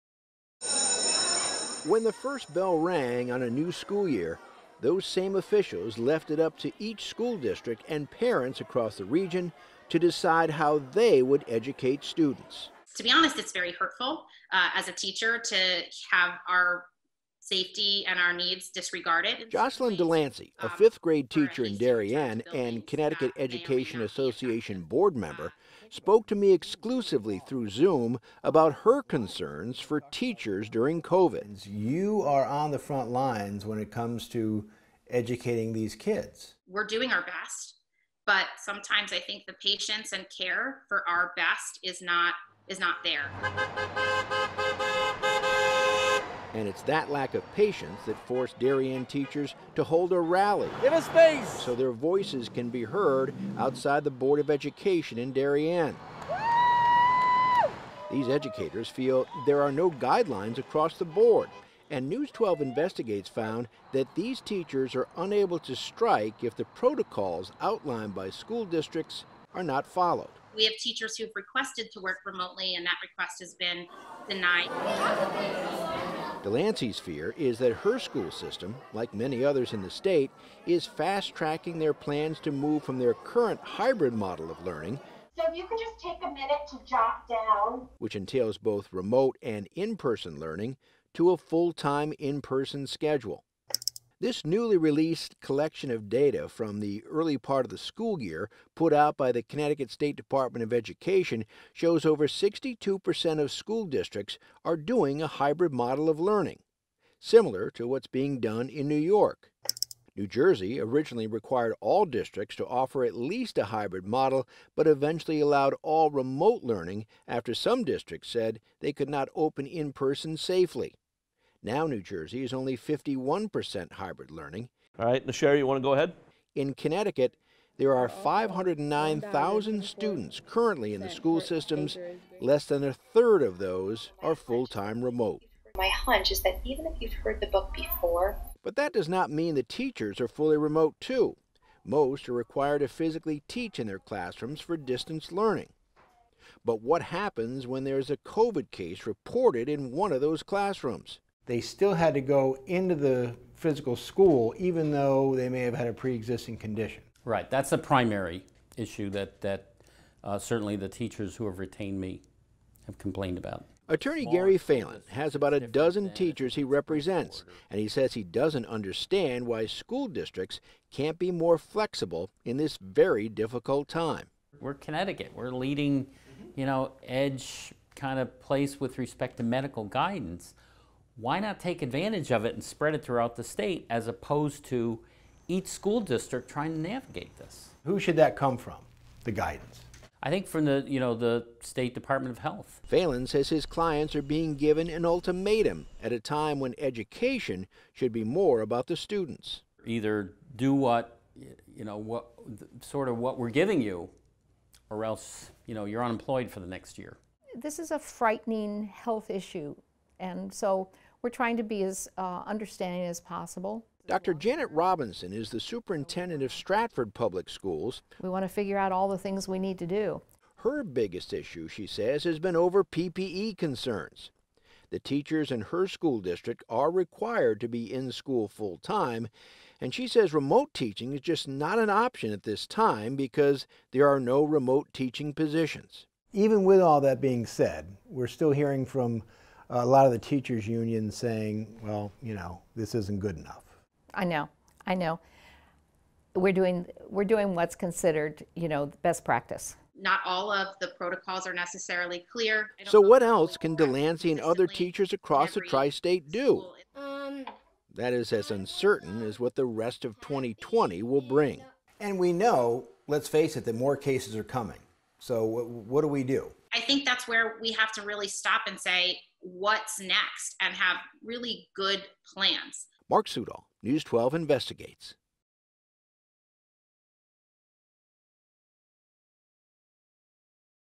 How are teachers balancing the new changes and their safety?
Children aren't the only ones affected during this time. News 12's Mark Sudol dives into teachers who are struggling to juggle the new school year and their safety amid their pandemic.